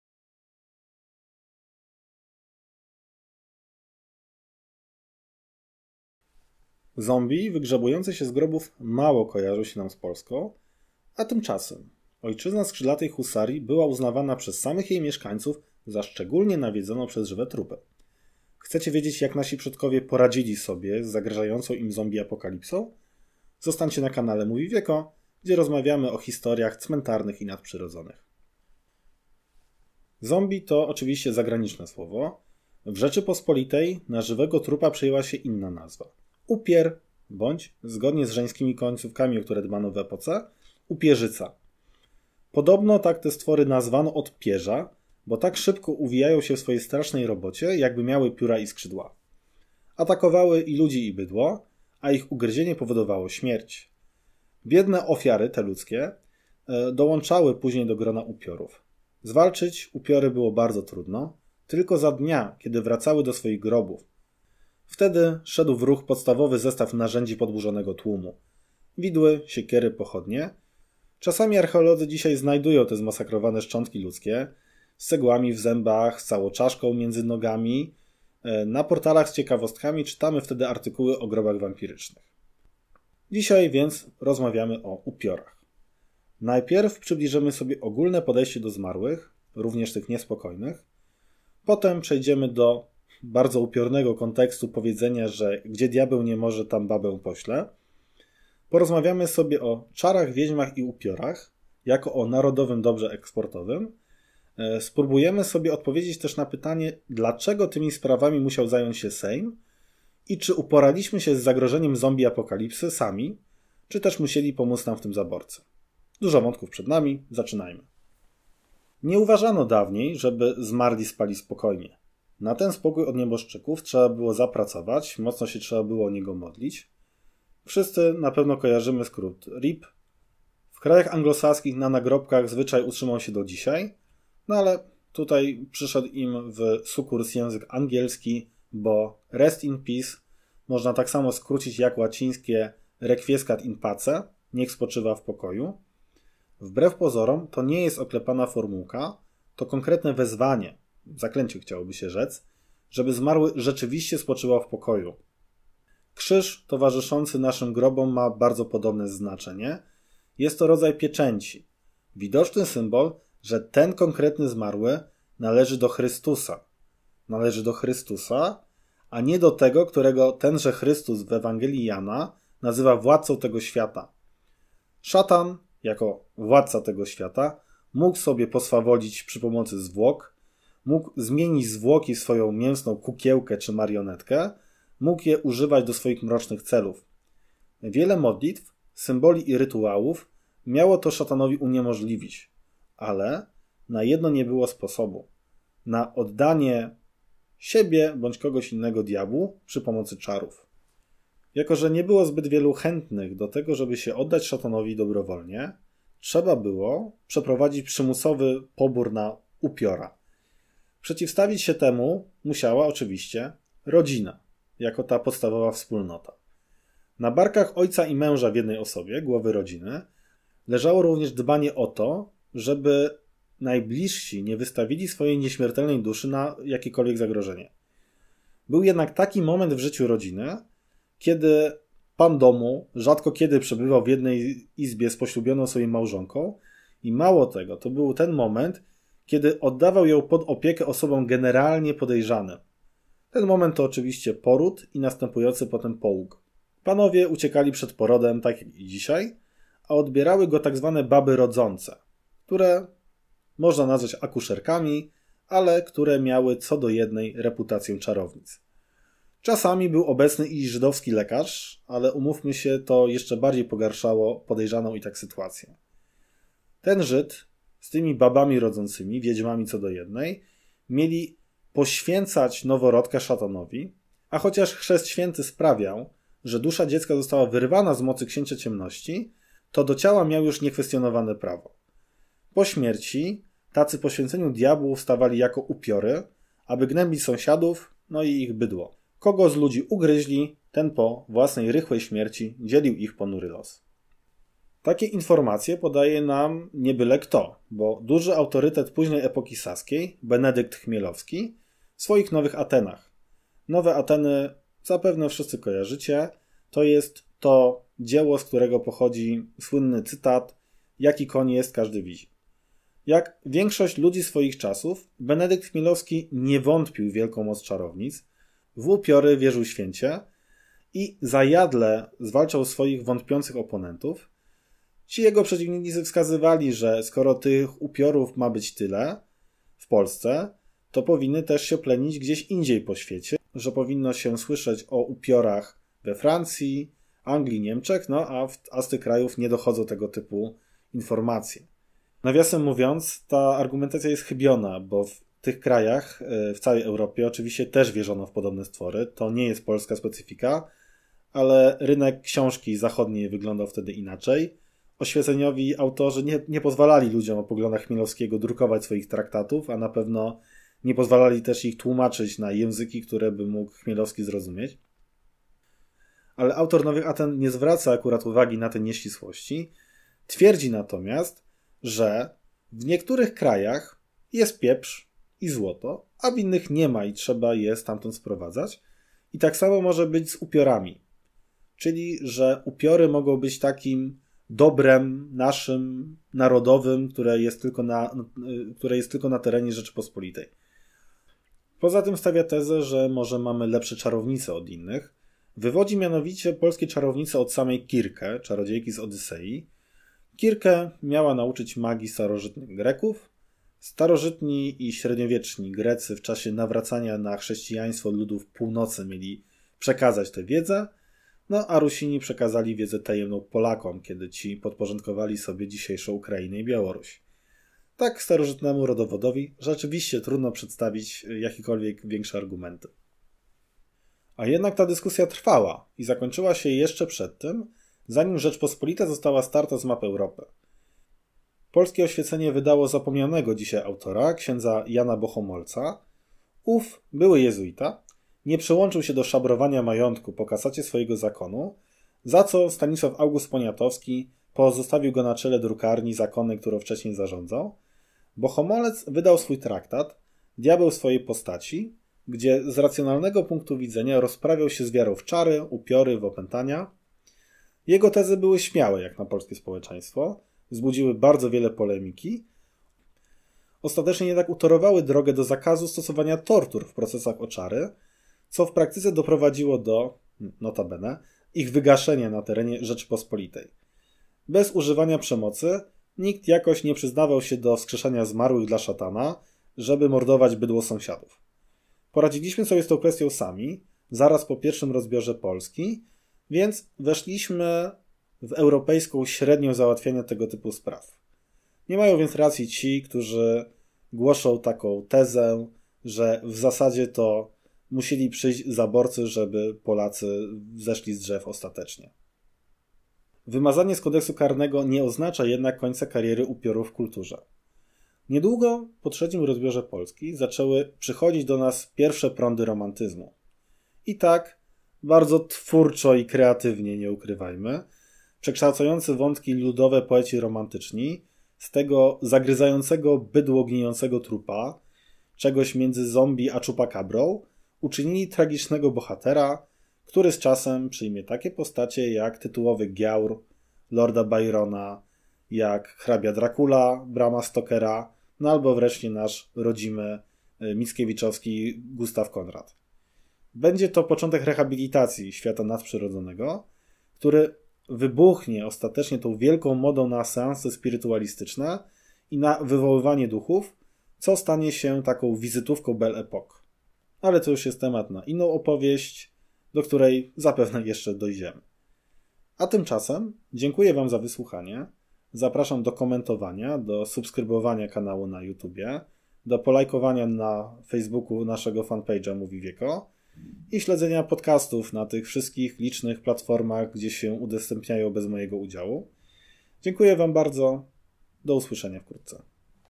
Upiory - staropolska zombie apokalipsa?
Zombie wygrzebujące się z grobów mało kojarzą nam się z Polską. Tymczasem, ojczyzna skrzydlatej husarii była uznawana przez samych jej mieszkańców za szczególnie nawiedzaną przez żywe trupy. Jak nasi przodkowie radzili sobie z zagrażającą im zombie apokalipsą?